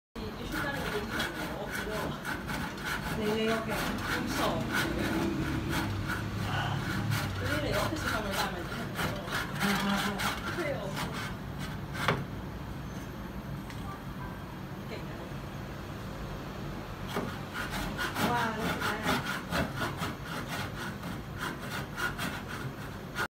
【kids woodwork】5 year old Jack is cutting wood
Many people are asking me what time what is a good age to start with woodwork.